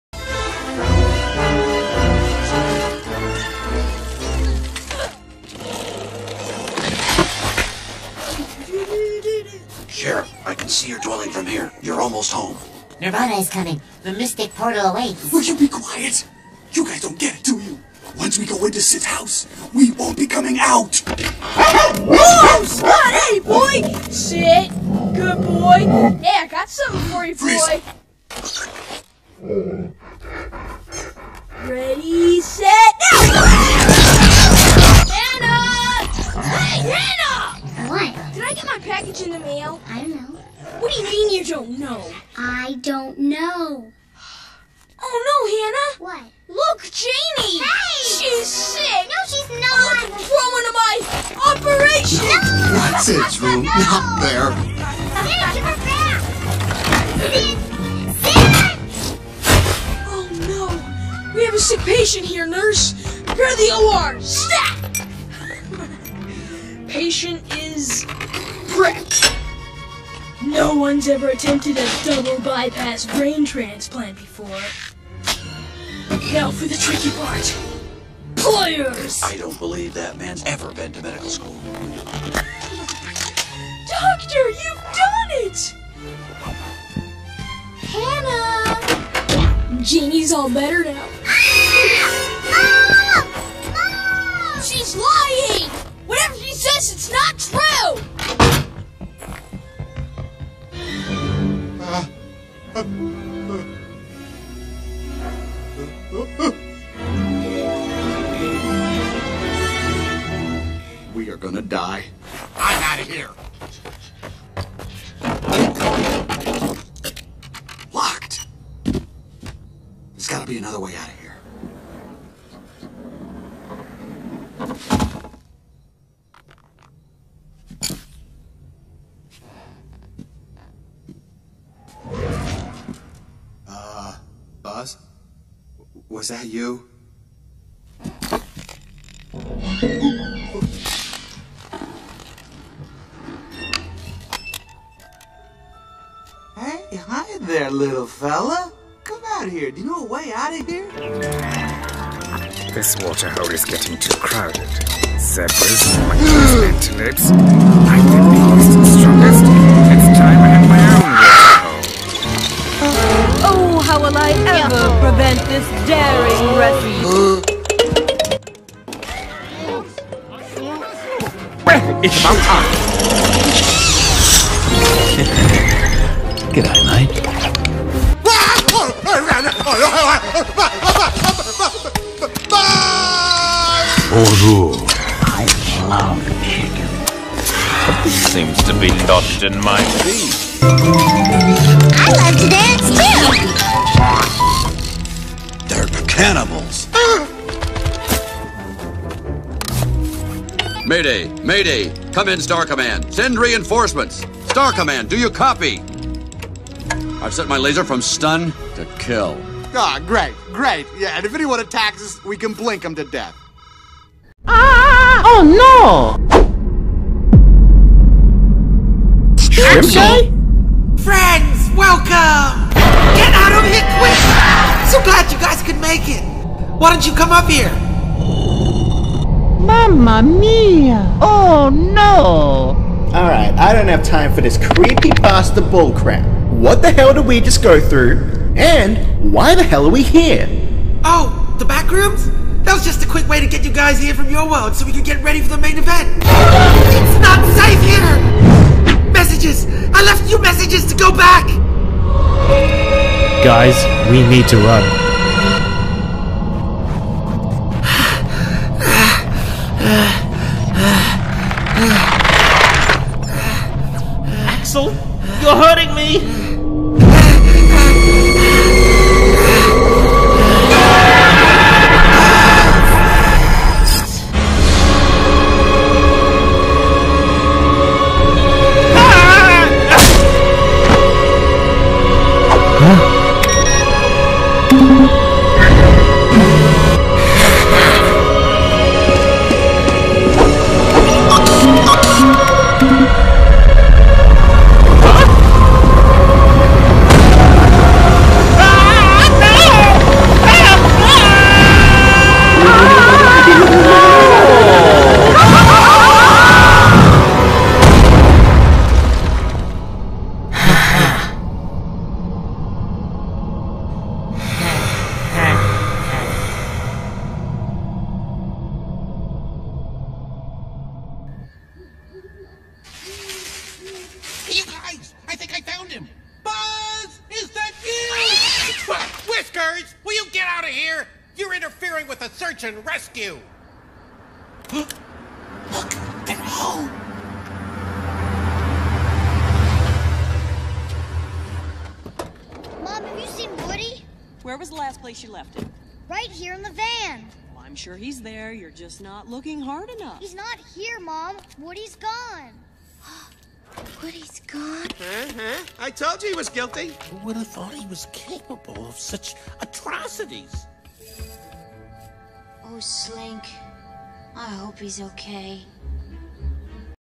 Word Party Story Part 10 - At Sonic.EXE's House/The Backrooms
Hello guys 👋🏻
My name Joshua Reyes Productions and This Video Word Party Story Part 10 - At Sonic.EXE's House/The Backrooms
Okey guys bye 😏👋🏻

Cast:

Franny as Woody

Henry Stickmin as Buzz Lightyear

Bob as Mr Potato Head

Fishy Boopkins as Slinky Dog

SMG4 as Hamm

Tari as Rex

Axol as Axol

Sonic.exe as Sid

Roblox Girl as Hannah

Bolt as Scud

Dave as Chosen Squeeze Toy Alien